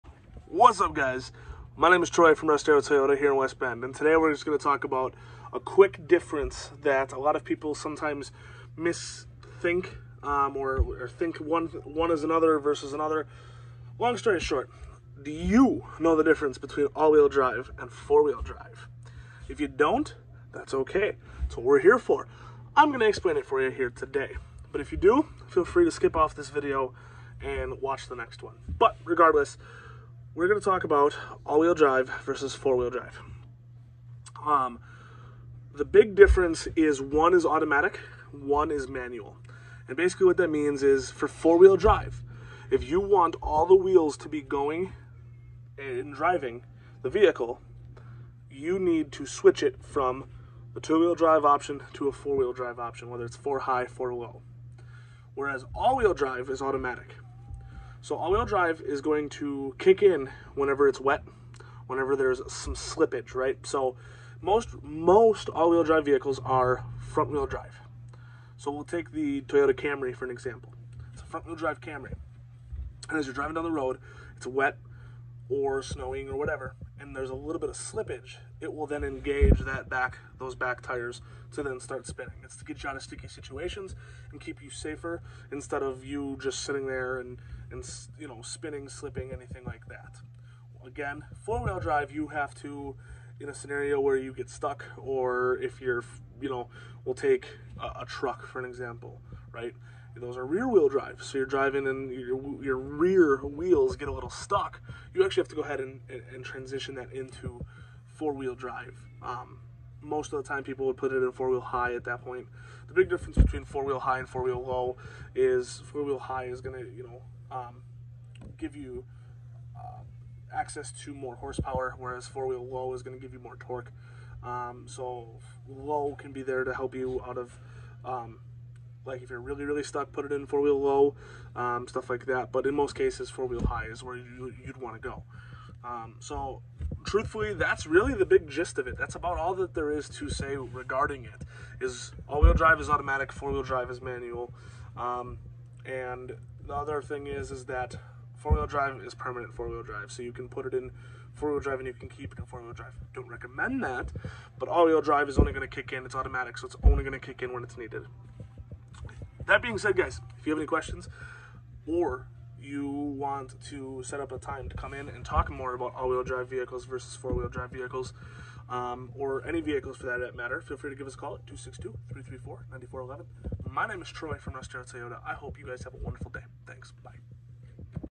AWD vs 4WD - Russ Darrow Toyota
Are you deciding between AWD or 4WD for your next vehicle? Join Troy from Russ Darrow Toyota as he goes over the differences between the two that may help with your decision!